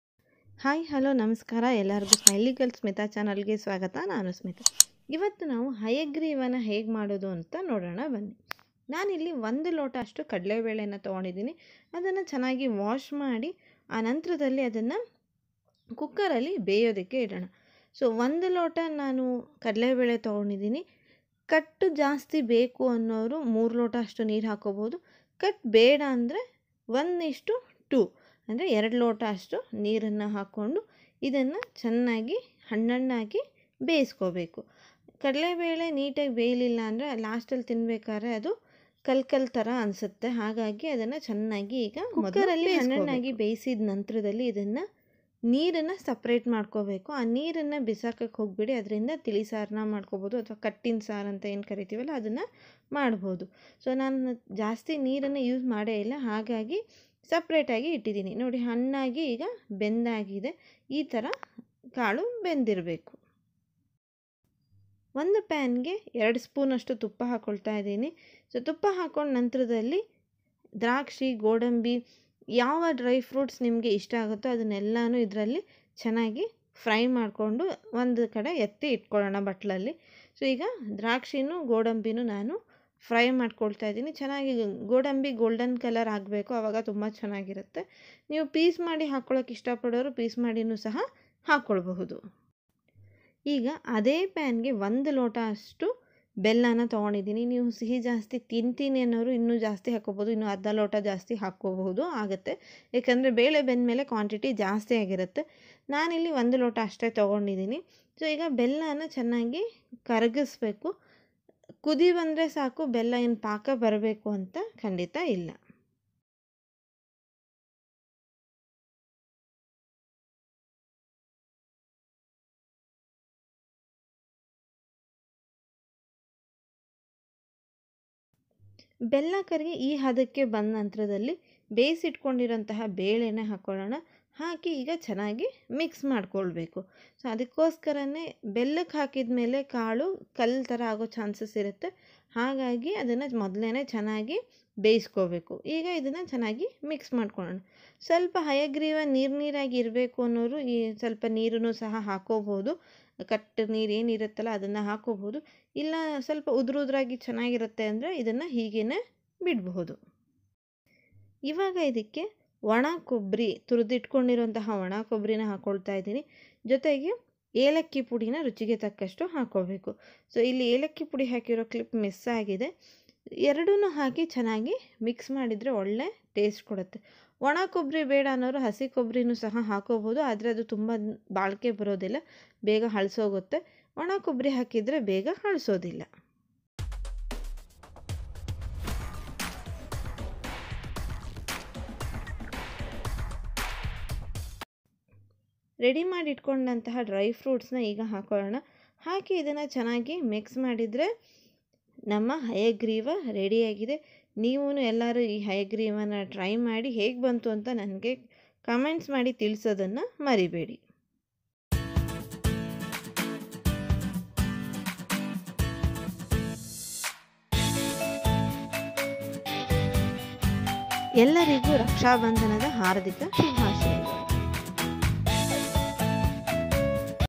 ಹಯಗ್ರೀವ | Hayagreeva recipe in Kannada | Hayagriva | Smileygirlsmitha
Hi friends,
In this video i m sharing Hayagriva recipe in Kannada.
Smileygirlsmitha

Thank u
Smitha